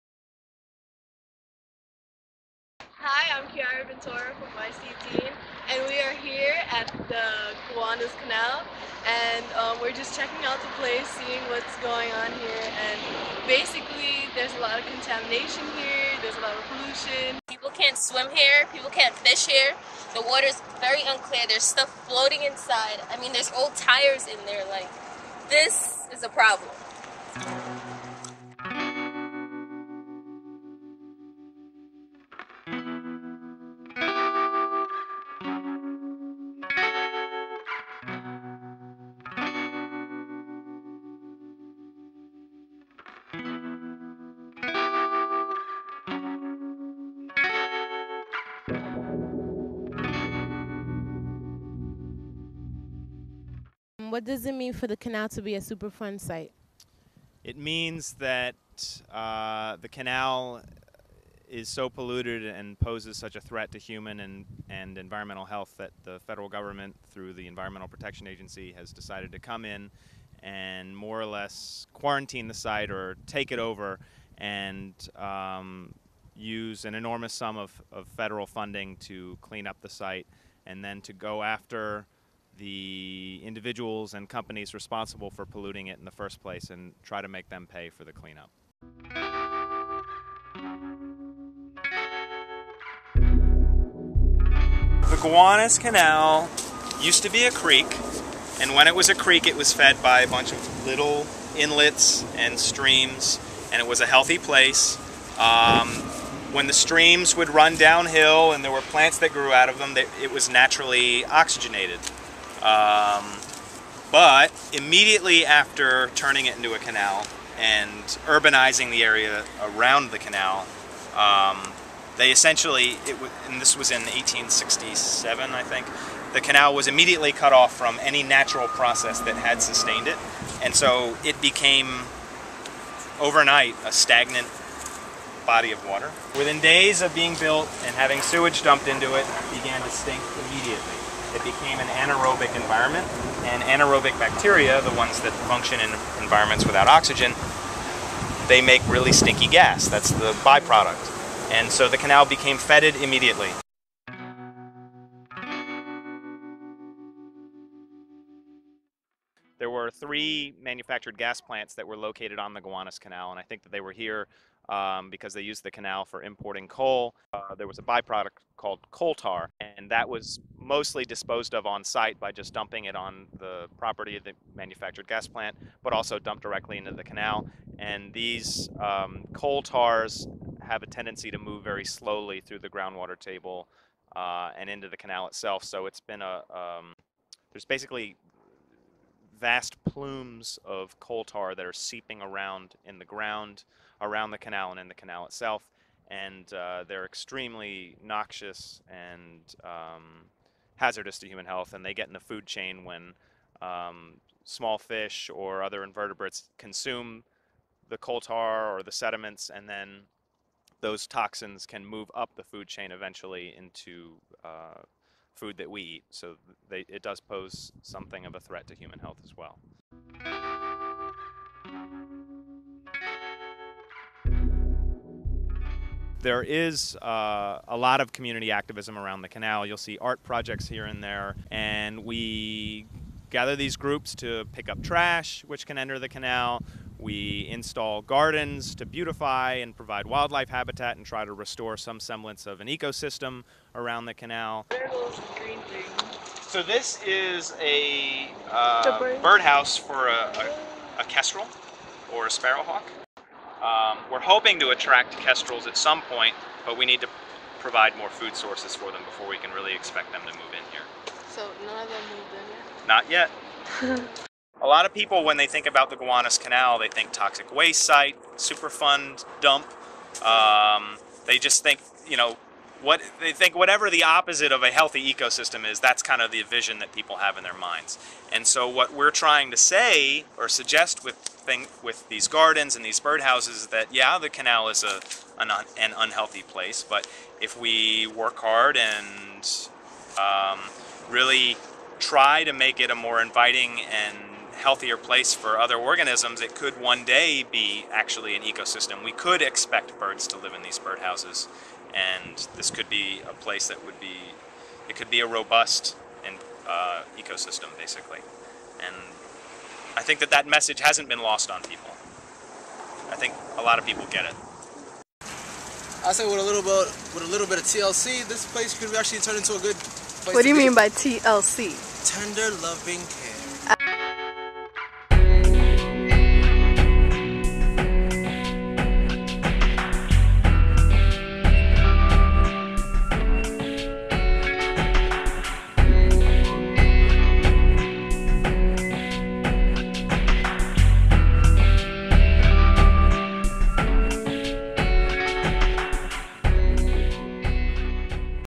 The Gowanus Canal: A Legacy of Pollution
YCteen reporters interview Hans Hesselein of the Gowanus Canal Conservancy and investigate the Superfund site themselves.